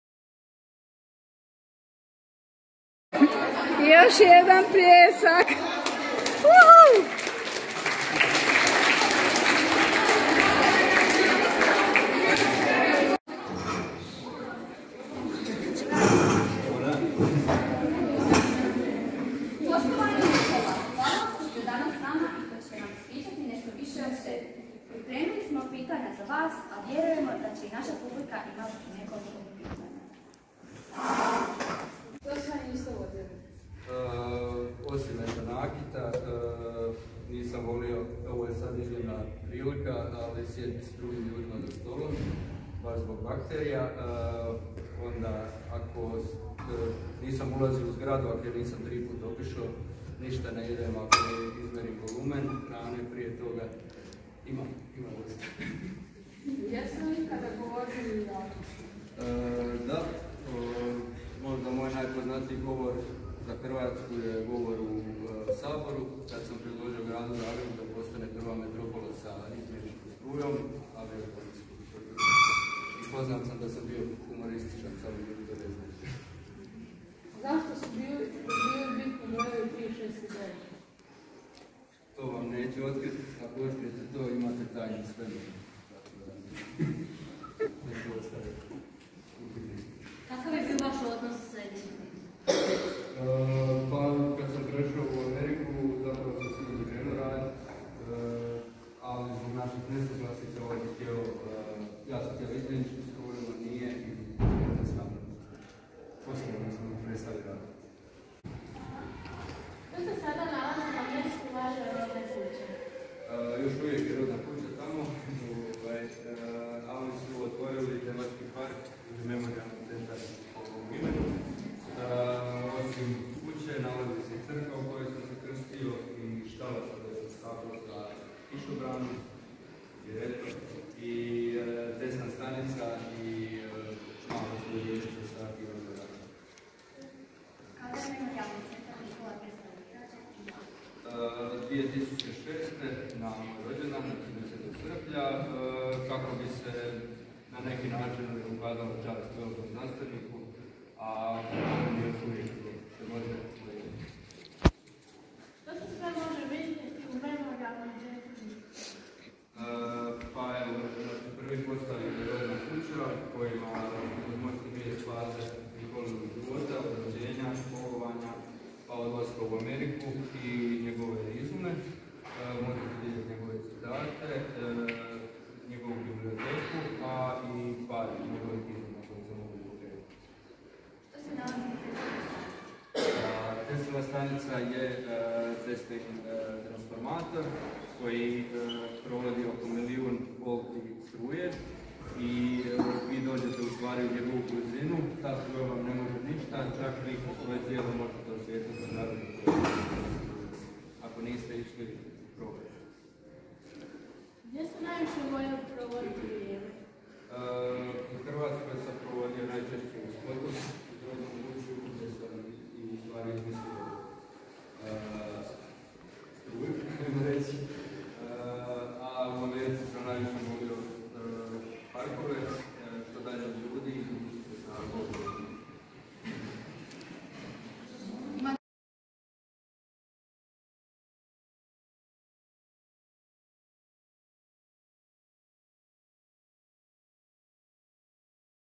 Intervju with "Nikola Tesla" - eTwinning project Hayata Yön Verenler 2 (Scientist)
eTwinning project Hayata Yön Verenler 2 (Scientist) - We arranged with a Tesla`s double to come to our school and introduce himself (Tesla, of course) in front of all pupils of 6th grade. It was really great experience. The Tesla`s double (Mario) acts like Tesla in events in our region about Tesla`s life and work. It was amazing to listen how students asked "Tesla" about everything they learned in memorial center and how "Tesla" was great in imitation. The students in audience, just like coleagues in school - Ana, Đurđica i Andriana - had a lots of questions for "Nikola" and he bravely answered on many questions for 45 minutes. Big thanks to Mario Paral from eTwinnig team and teacher Željka.